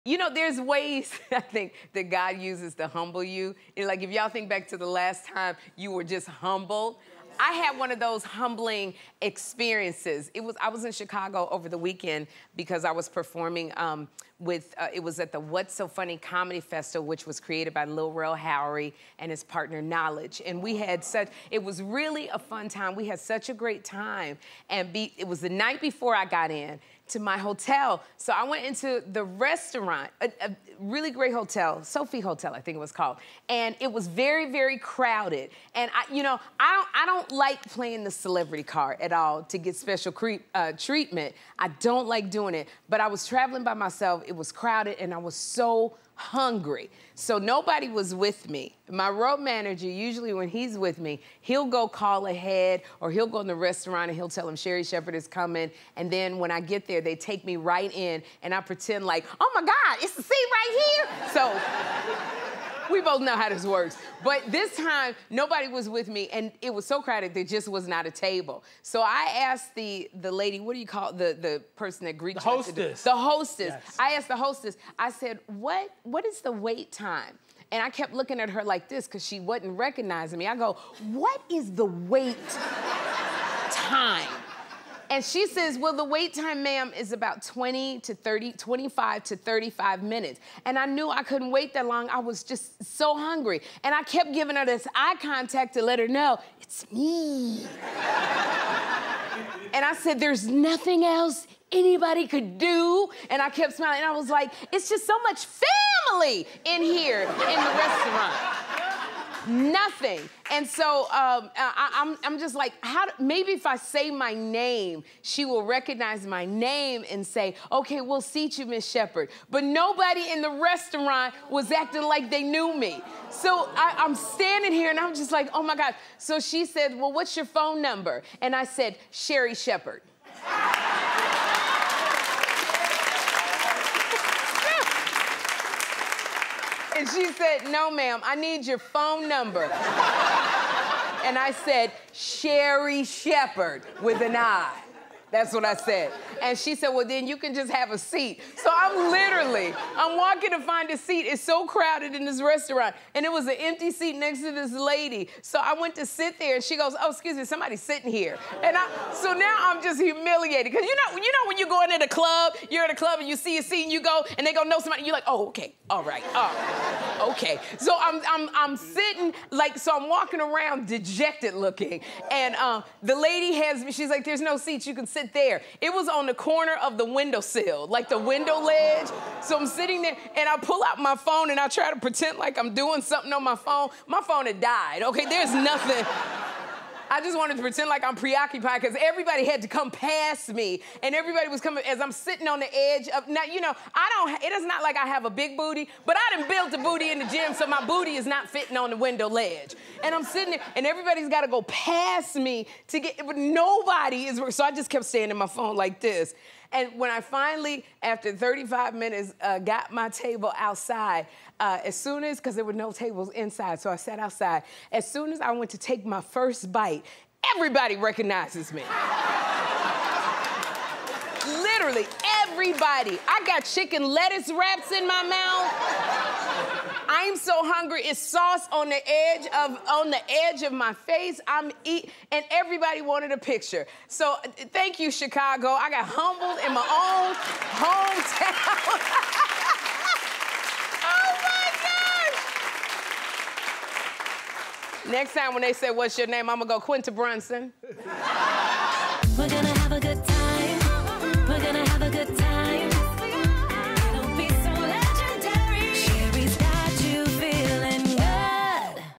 Trying to Skip the Line | Sherri Shepherd
Sherri tried to drop a name and skip the wait at a restaurant- but, well… 🤣